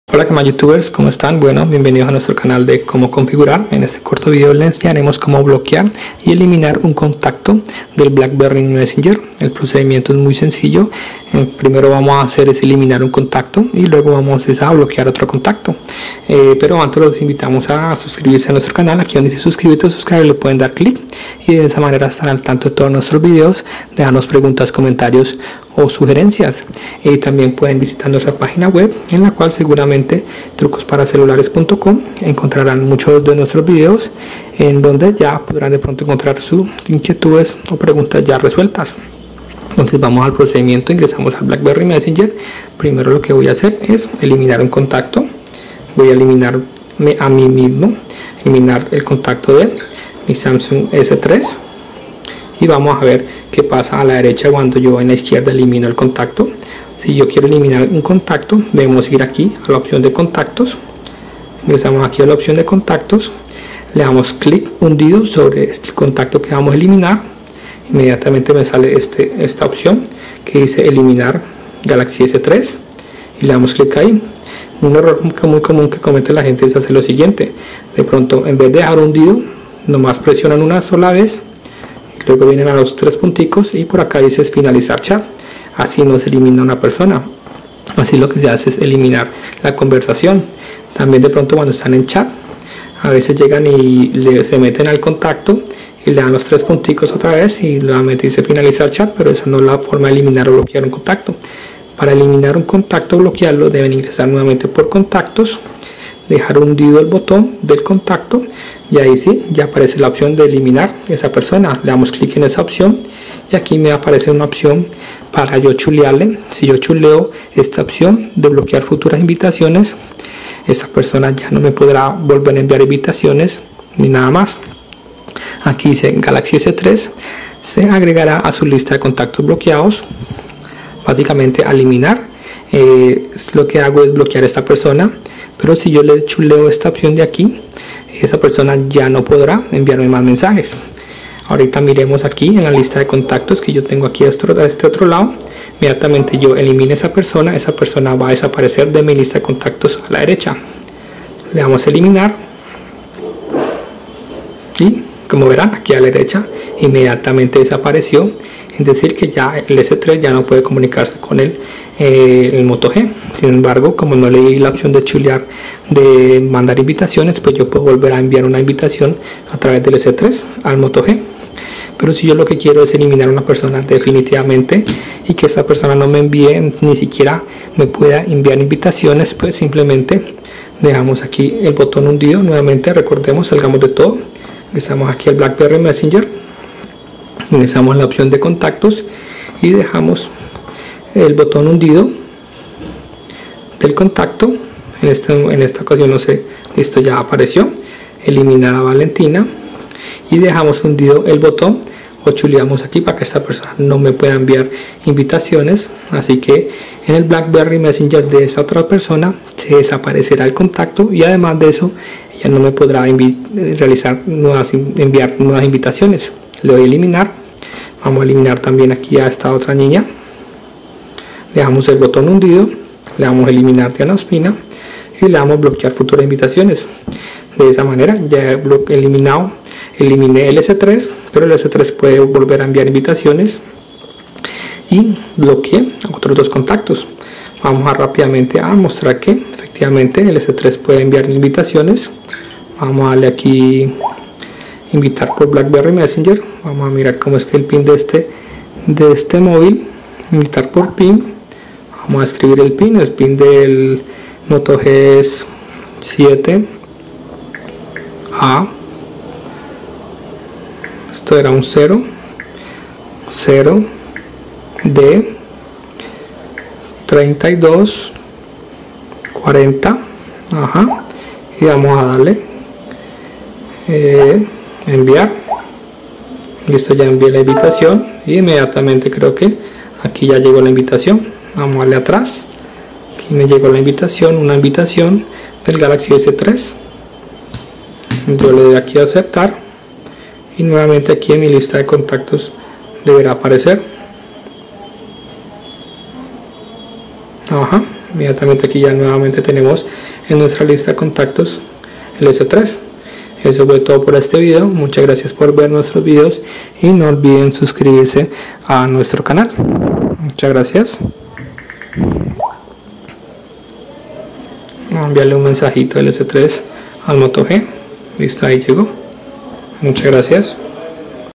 Cómo bloquear o Eliminar contacto del Blackberry Messenger Motorola Moto G X T1032 español
XT1032 Motorola Moto G Cómo bloquear y desbloquear los contactos de BlackBerry Messenger Android
Trucos XT1032 Motorola Moto G